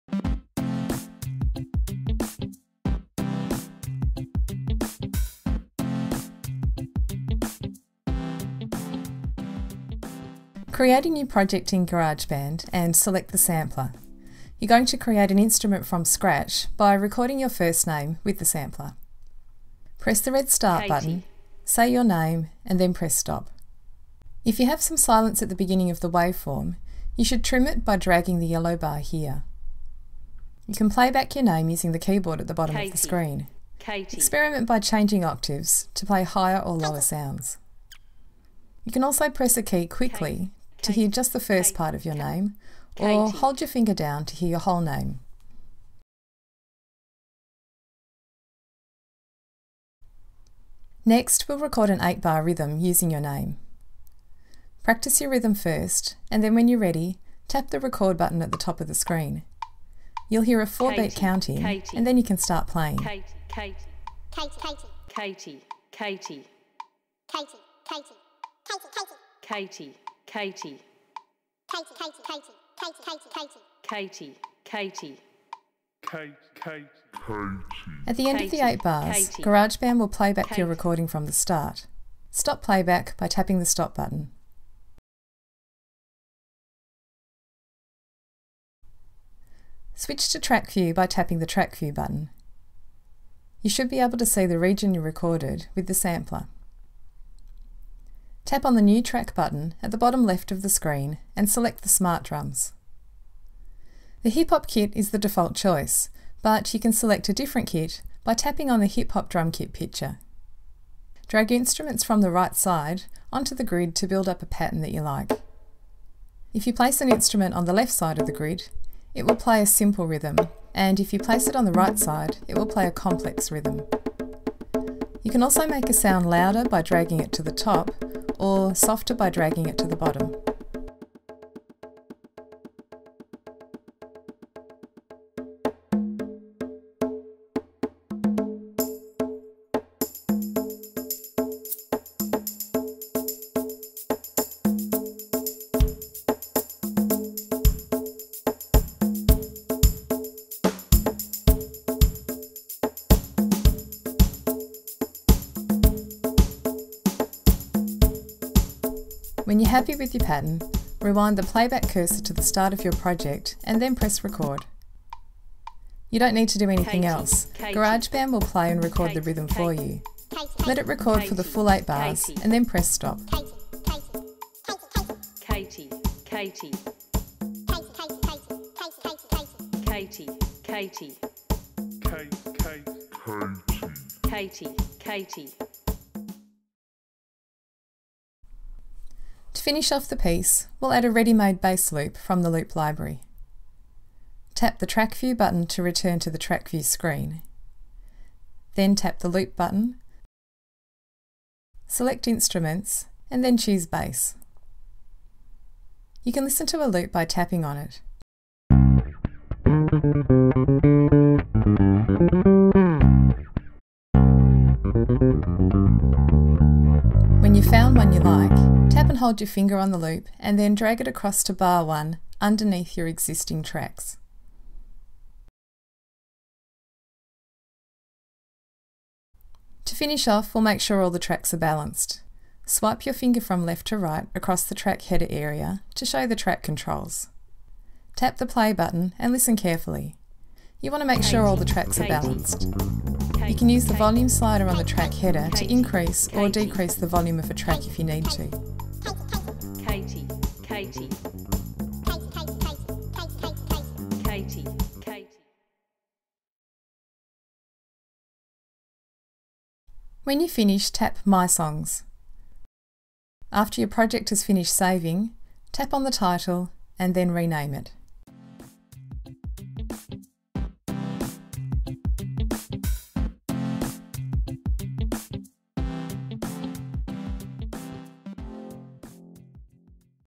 GarageBand for iPad Project for Students Using The Sampler
This project is from the iPad Projects in the Music Classroom ebook
A simple hands-on iPad project you can do with students using the GarageBand app on iPad.